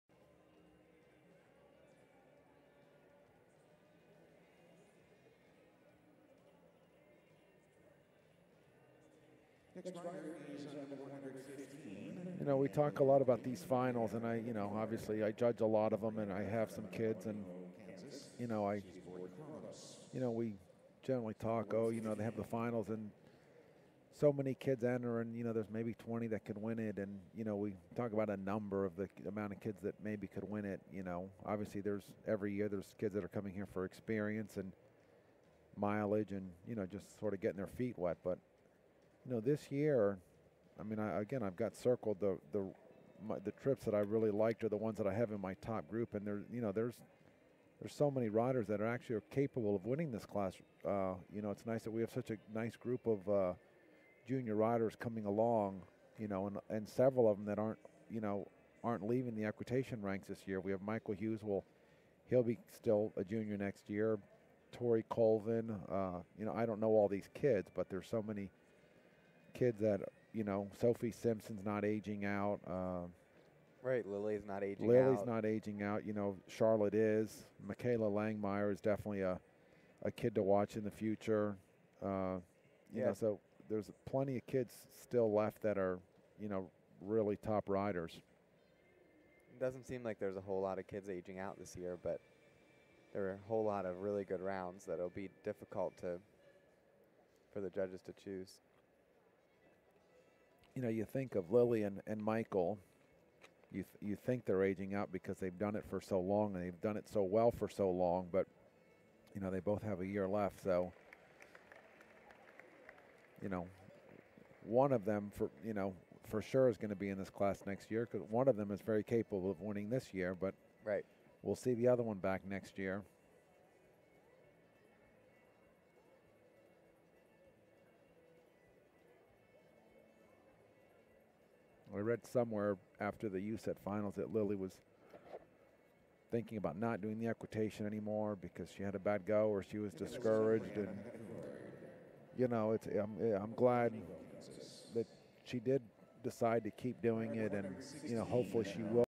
115 Brianna Peddicord, Maclay Finals First Round
Maclay Finals - First Round, from the Alltech National Horse Show, November 3, 2013.